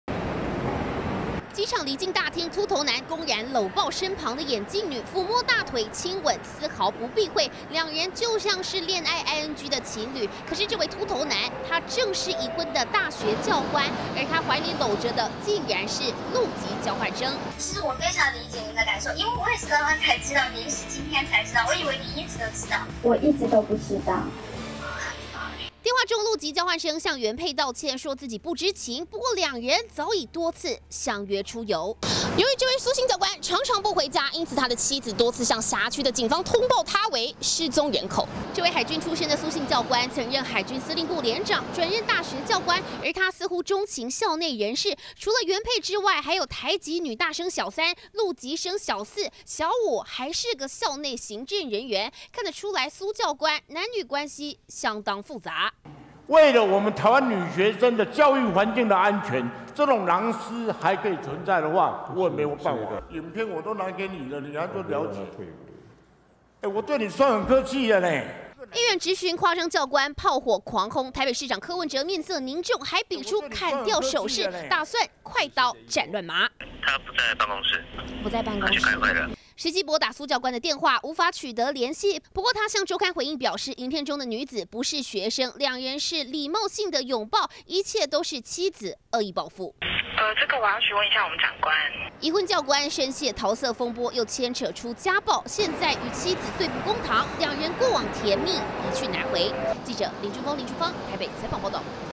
20190531中天新聞 已婚教官曖昧陸籍交換生 機場親暱擁抱
台北市某大學已婚教官，被議員爆料，竟然與陸籍交換學生有曖昧關係，被直擊在機場，有親密舉動，甚至傳出跟其他女學生，也有不單純的關係，柯文哲看到影片臉色凝重，比出砍掉的手勢，要這名教官退休。